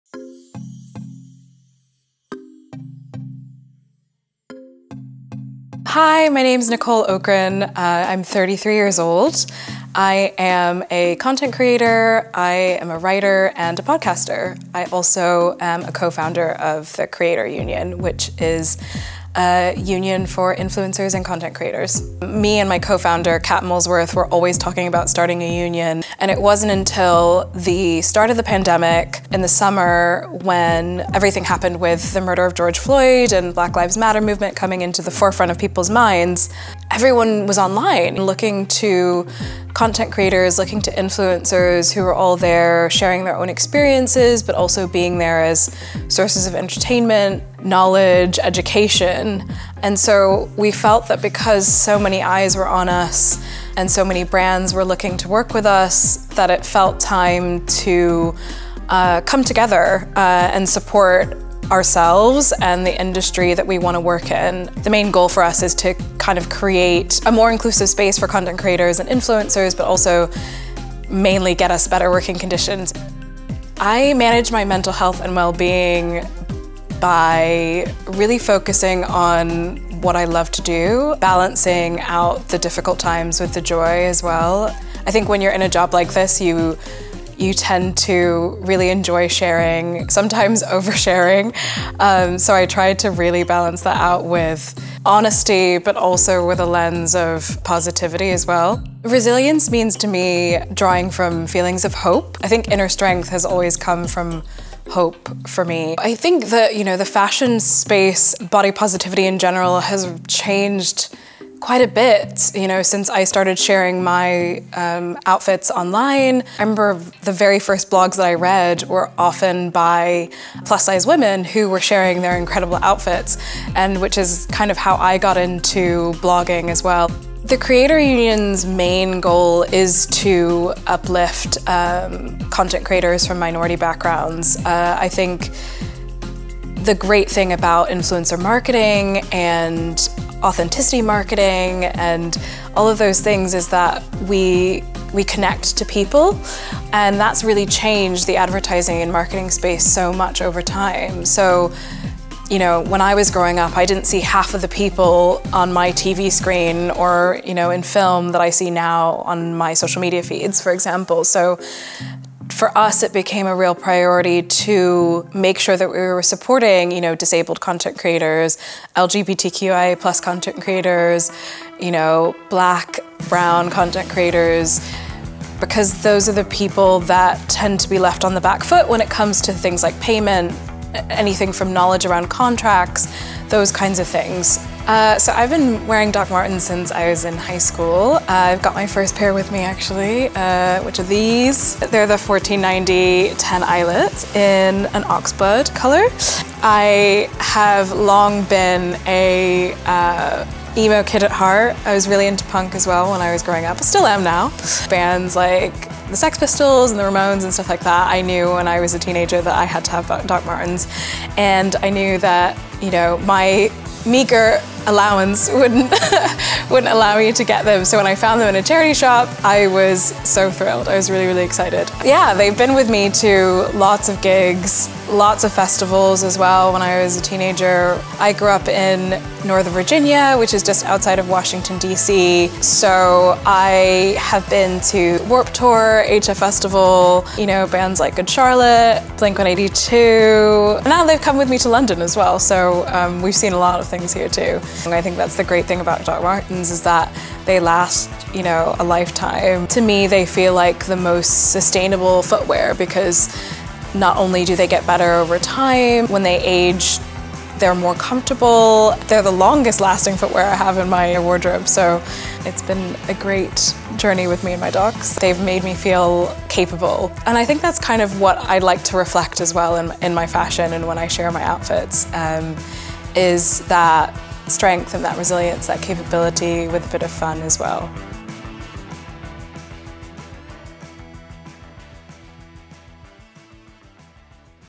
Resilience | Country Attire x Dr Martens with Nicole Ocran
We catch up with digital content creator and co-founder of The Creator Union, Nicole Ocran as we explore how her personal style has evolved along with her trusty Dr Martens boots, the resilience needed to build a successful career in the influencer space, and how she uses her platform to empower herself as well as other creators in the industry.

See more from Country Attire